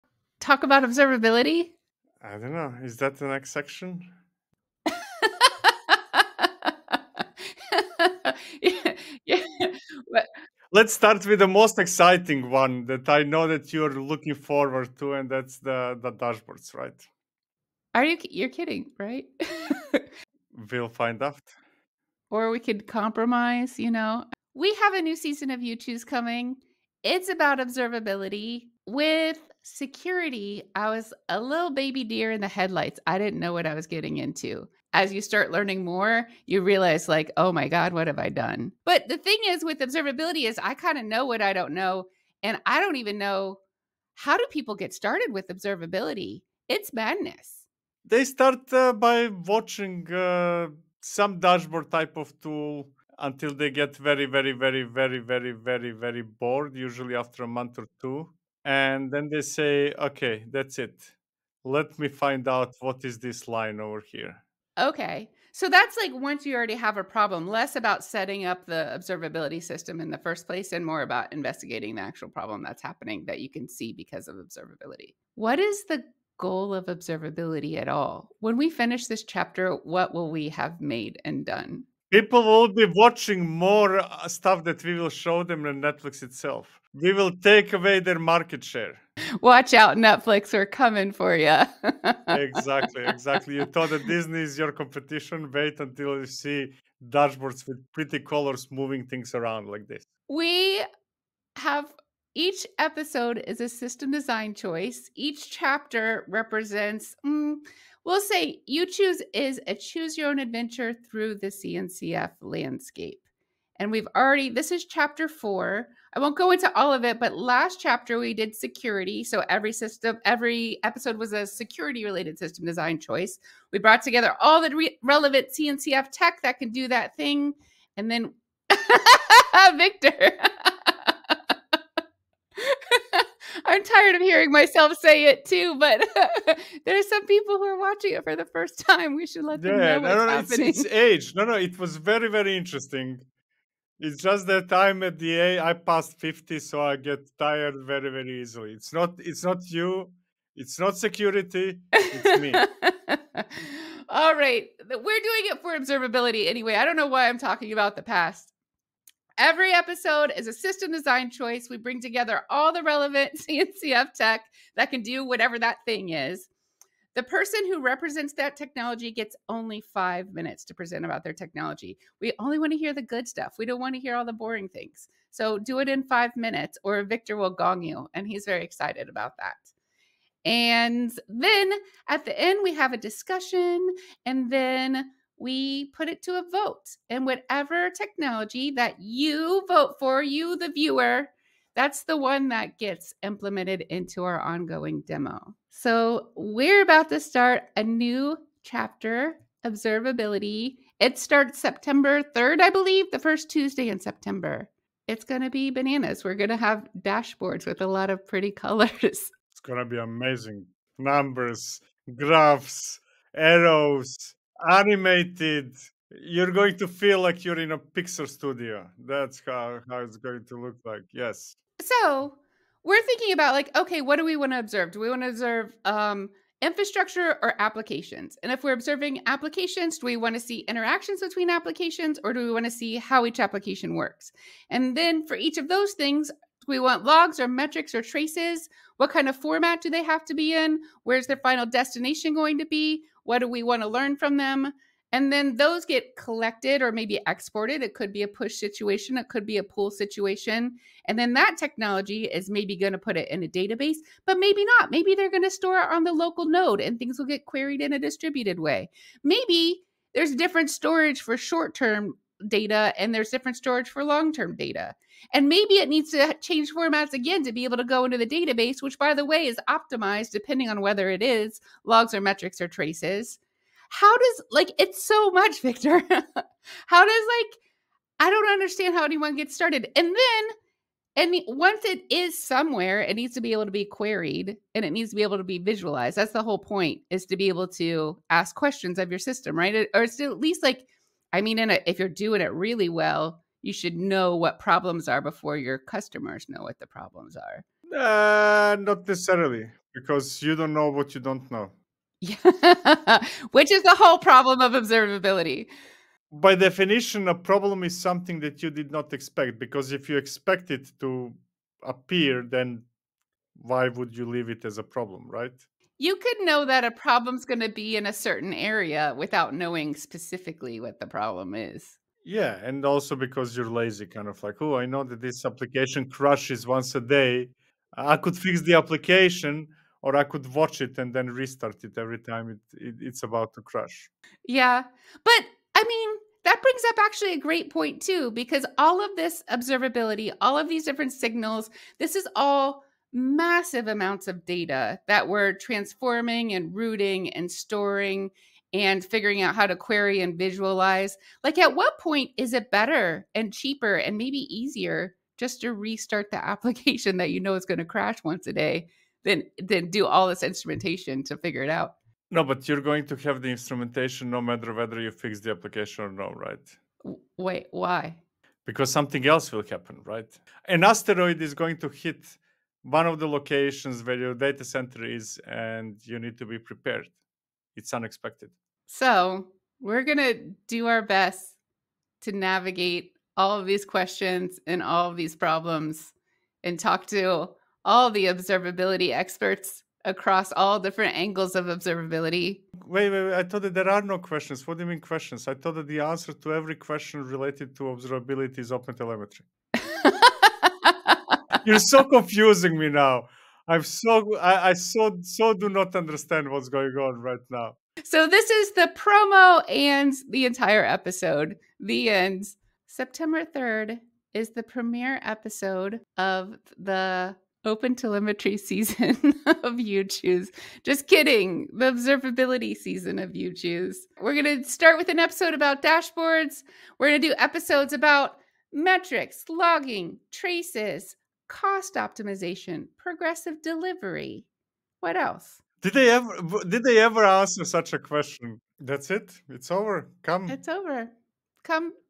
The Observability Odyssey (You Choose! Ch. 4, Ep. 0)
Chapter 4 of "Choose Your Own Adventure" is about to begin! In this one, we'll explore observability tools among CNCF projects.

More information about the "Choose Your Own Adventure" project including the source code and links to all the videos can be found

▬▬▬▬▬▬ 💰 Sponsoships 💰 ▬▬▬▬▬▬
If you are interested in sponsoring this channel, please use to book a timeslot that suits you, and we'll go over the details.